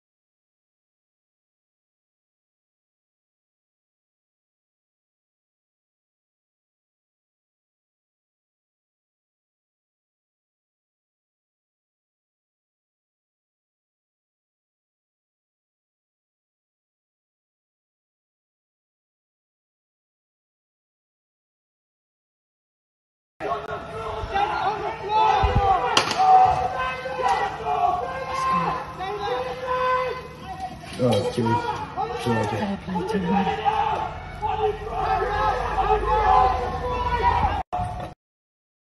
UK/Birmingham incident - Mad Man with a Machete in Castle Vale - beware (September 2022)
@Dublin Menace

@95 CRIP
chav Carlisle chavs Carlisle, chav botcherby carlisle  chavs fight fighting  gang crew,